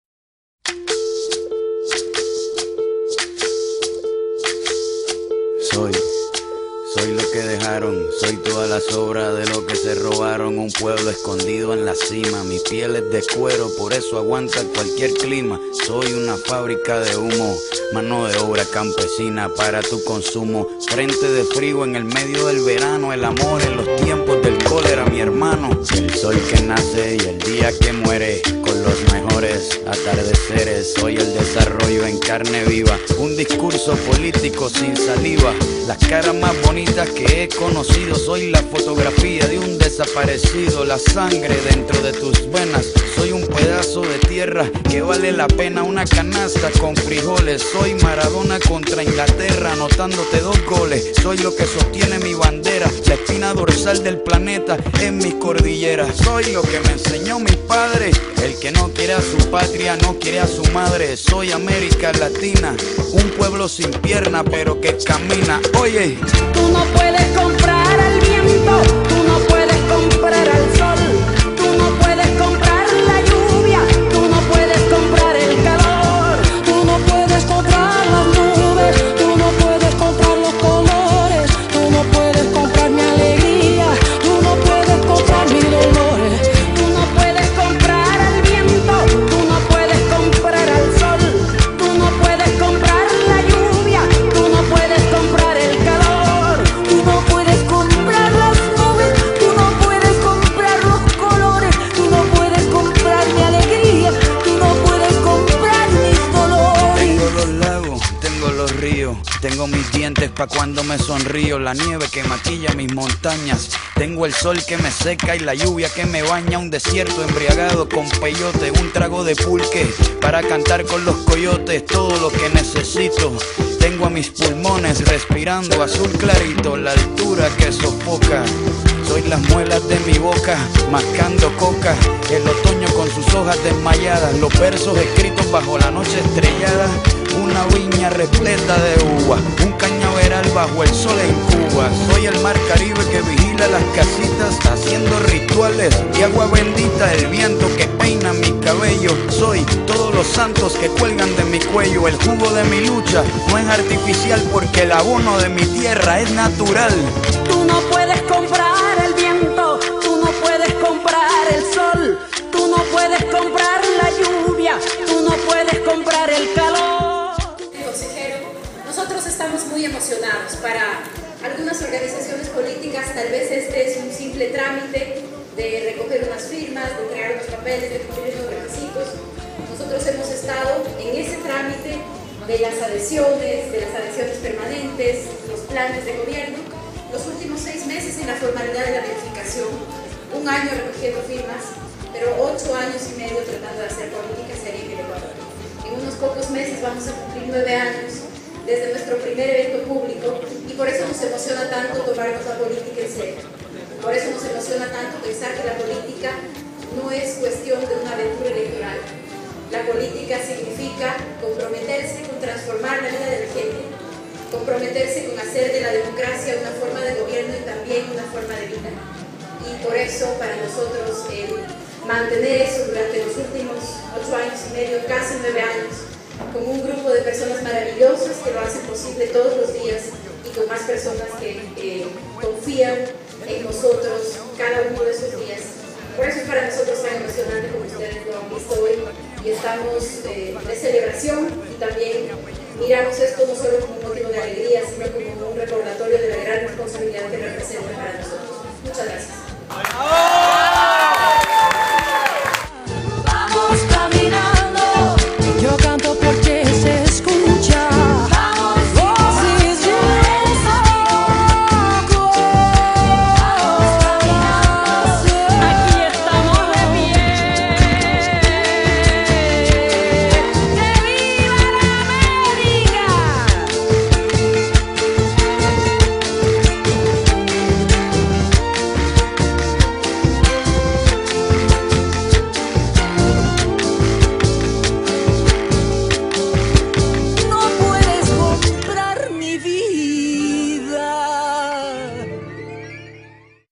Nuestra historia - R25
Un recorrido por los momentos clave de la historia de Ruptura 25.
Incluye el discurso de María Paula Romo el día de nuestra inscripción como Movimiento Político Nacional: "La política no es cuestión d